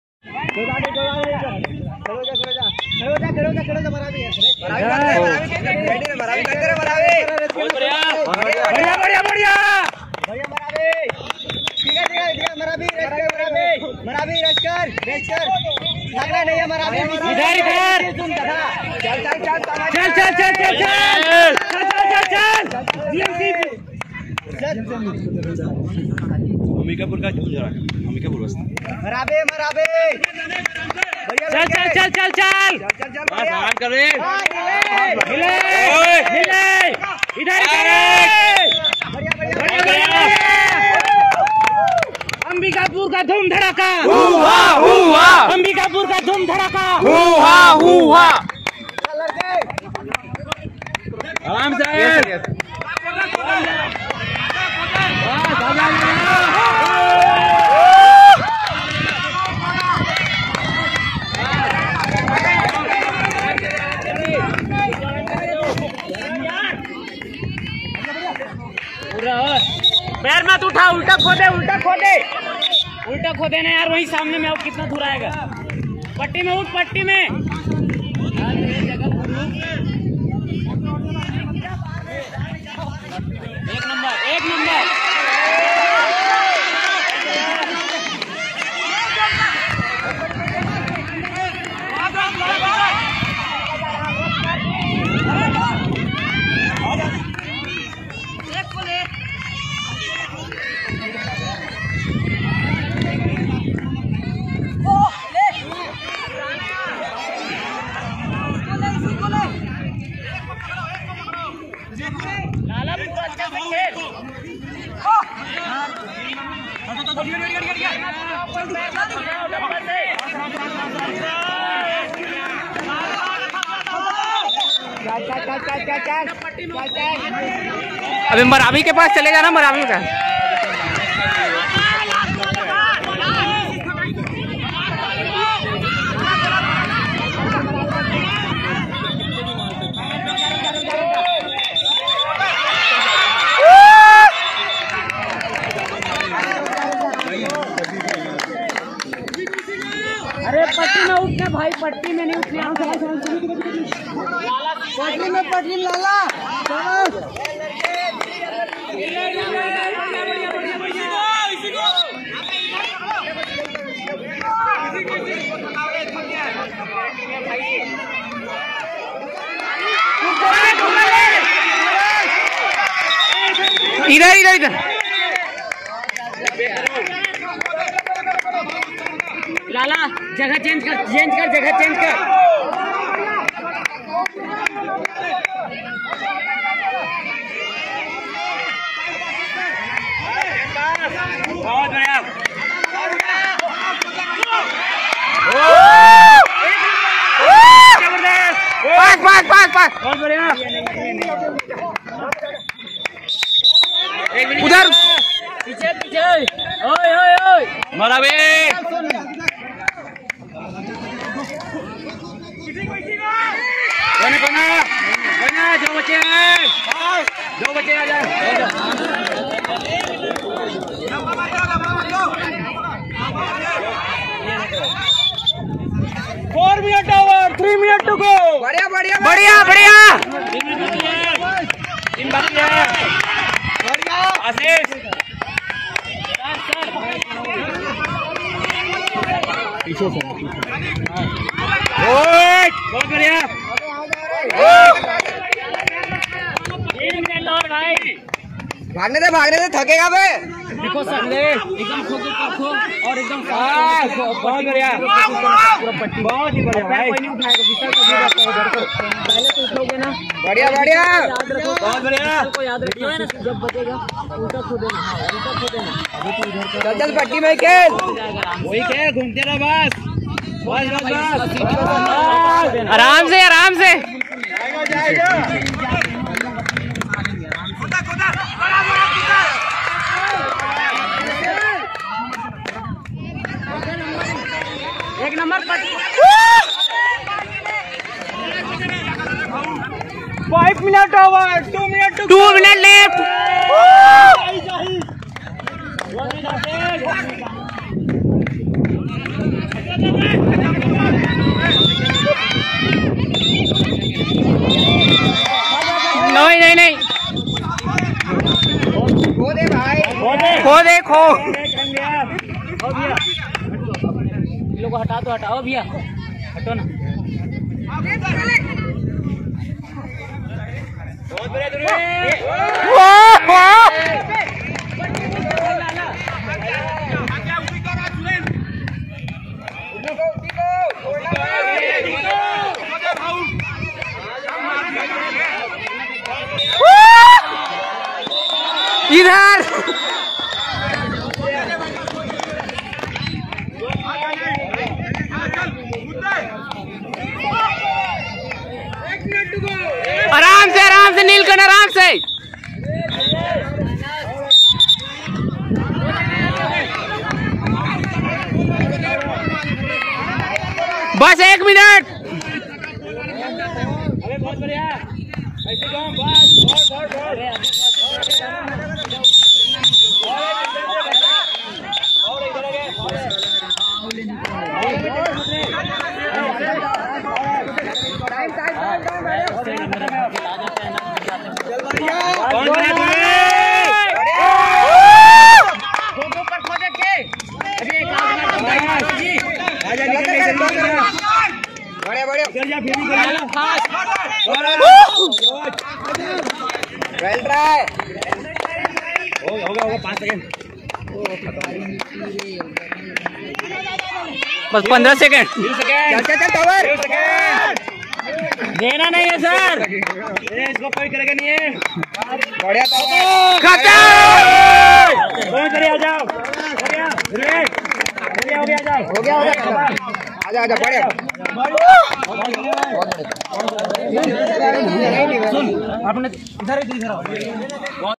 Intercollege Kho Kho Match Gmc Ambikapur vs Shankaracharya nursing college 💪💪
vlog gmc Ambikapur
Neelkanth Nilli Dhurve
Intercollege Kho Kho Match
Intercollege sports in medical College